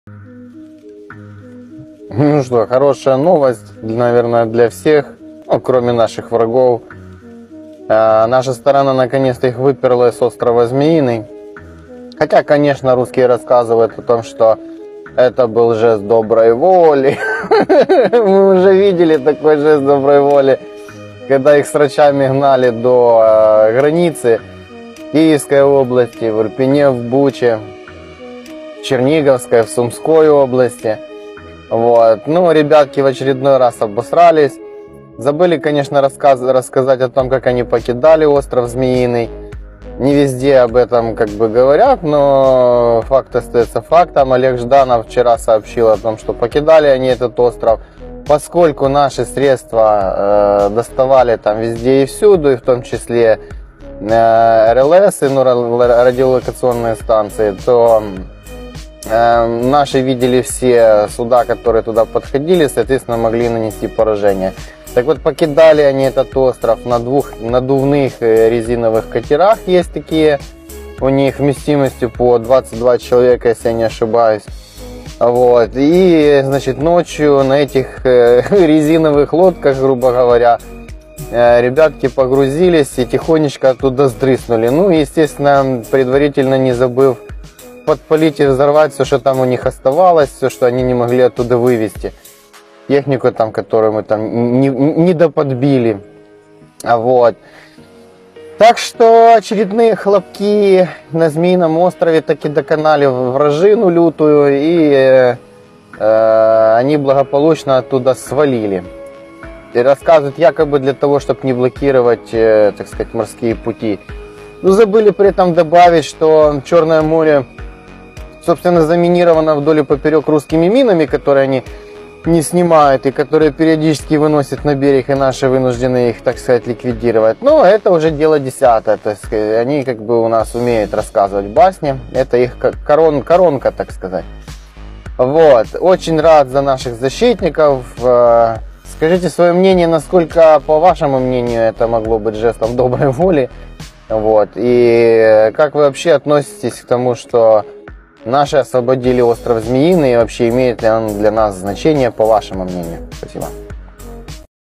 ОЧЕРЕДНОЕ ПОРАЖЕНИЕ РОССИЙСКИХ ВОЙСК В УКРАИНЕ
Русские войска "благодушно" оставили остров Змеиный, так ли это?українськийютуб